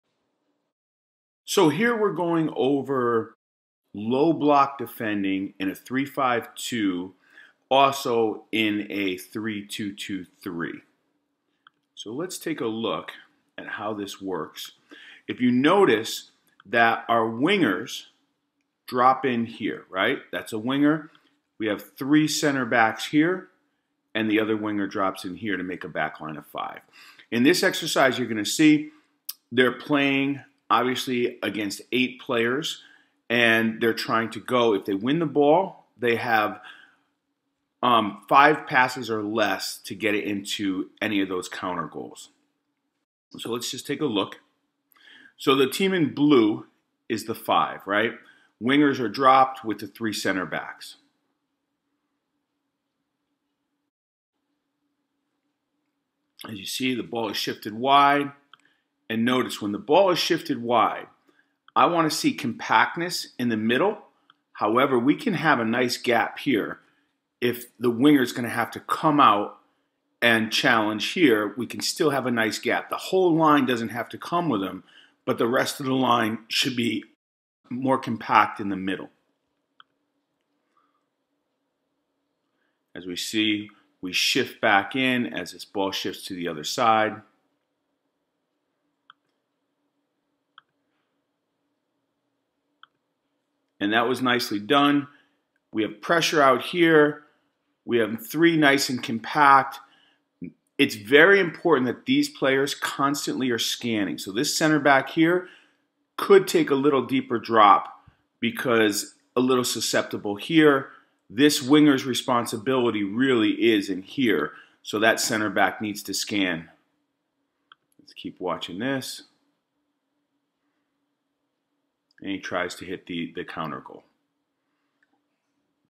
Soccer Strategy: Defending the Low Block in 3-5-2 and 3-2-2-3 Formations!
with Marcus DiBernardo,
Monroe College Head Men's Coach;
2019 NJCAA Division I National Champions (18-0);
2018 NJCAA Division I Women's National Champions (16-0-1);
6x NJCAA National Tournament appearances;
2019 United Soccer Coaches Men's Soccer Coaching Staff of the Year;
2018 United Soccer Coaches Women's Soccer Coaching Staff of the Year;
2012 NSCAA Coach of the Year

Gain a blueprint to understand and implement the 3-5-2 and 3-2-2-3 formations with your squad!

Create an attacking game model and build out of the back through numerical advantages and spacing!

Understand player roles and responsibilities, plus how to identify and correct breakdowns in coverage!

Discover how Coach DiBernardo "splits" the field and how this affects defensive positioning!

Championship Productions - the world's #1 source for quality coaching instruction since 1976!